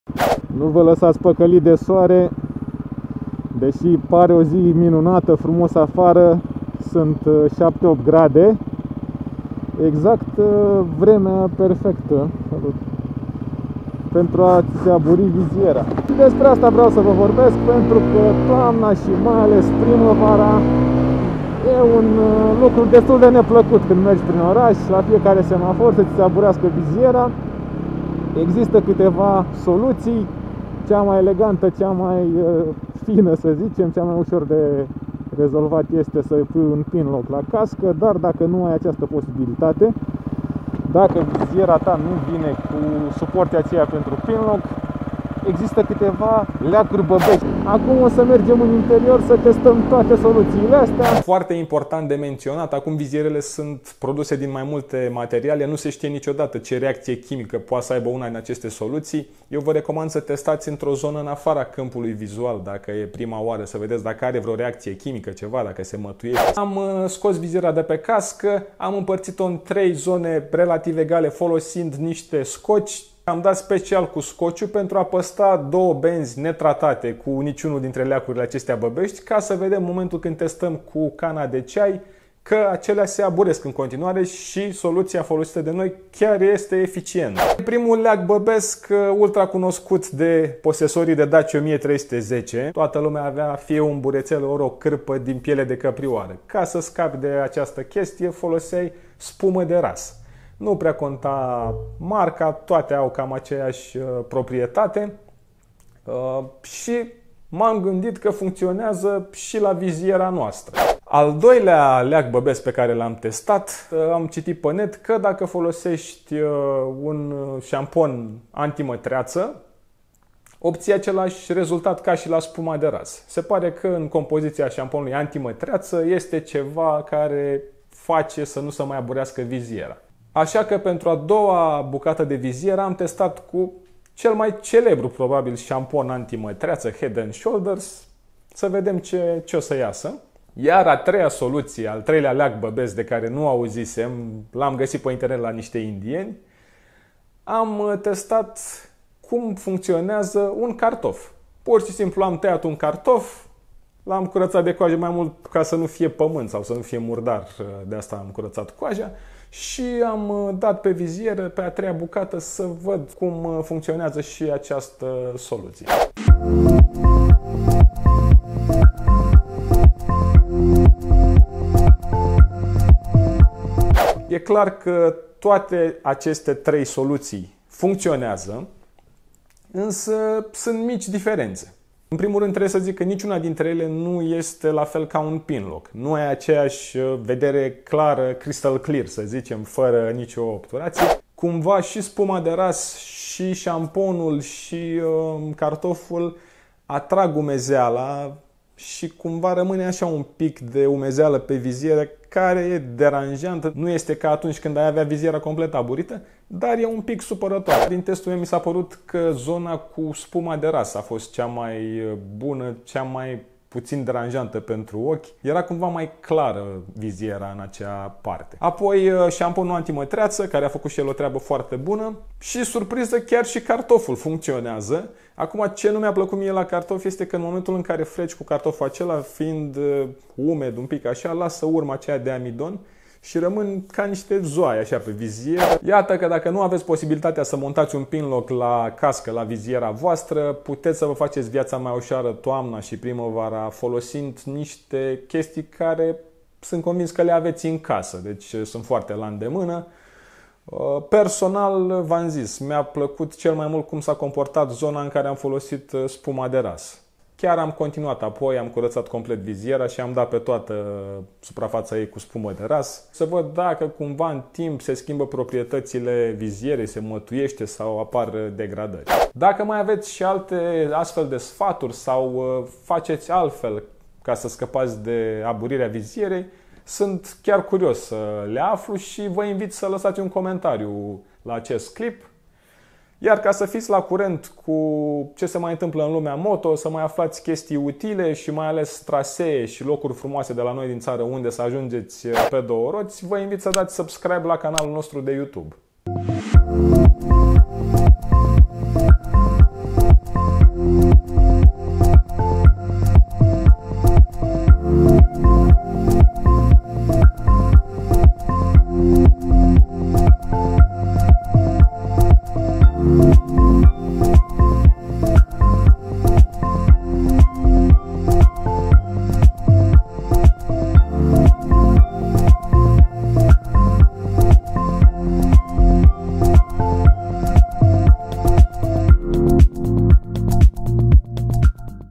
moto Tips'N'Tricks : Cum scăpăm de viziera aburită ? De la cartof la Pinlock.
Unul din lucrurile neplăcute și chiar periculoase, ale mersului cu motocicleta, este aburirea vizierei.
Acest lucuru se întâmplă cu precaderea primăvara și toamna/iarna, când temperatura externă este mai scăzută, dar se poate întâmplă și în mijlocul sezonului, vara, în diminețile răcoroase sau în timpul unor ploi puternice.
Soluția cea mai elegantă și eficientă pentru a scapă de această problema, este folosirea unui Pinlock.
Dar cum nu toate căștile/vizierele sunt prevăzute cu sistemul de prindere al Pinlockului, există și câteva "leacuri băbești" pentru a scapă de această problema: spumă de ras, șampon anti-mătreață și un banal cartof.